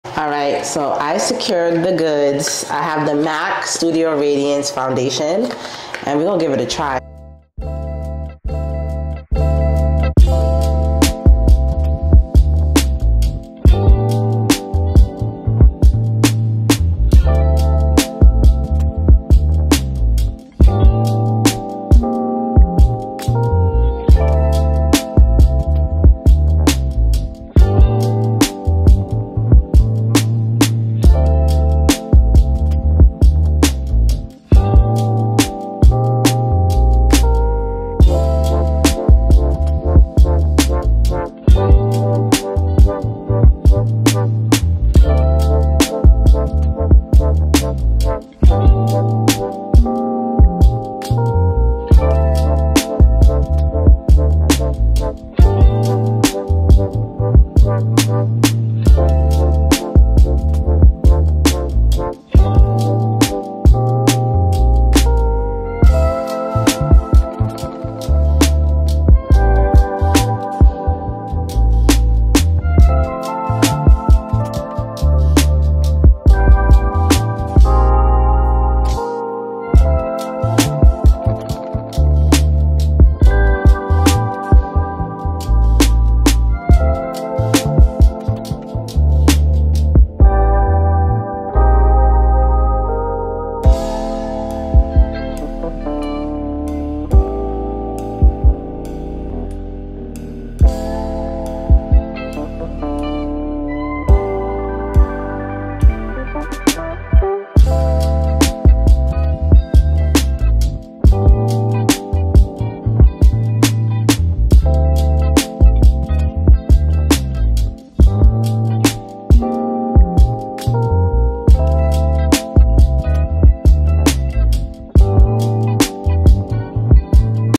Natural Makeup Look with the MAC Studio Radiance Foundation on Dark Skin-NW50
Testing out the new MAC Studio Radiance Foundation in NW50. I highly suggest you get color matched for this foundation. The shades are different from the Fix Fluid.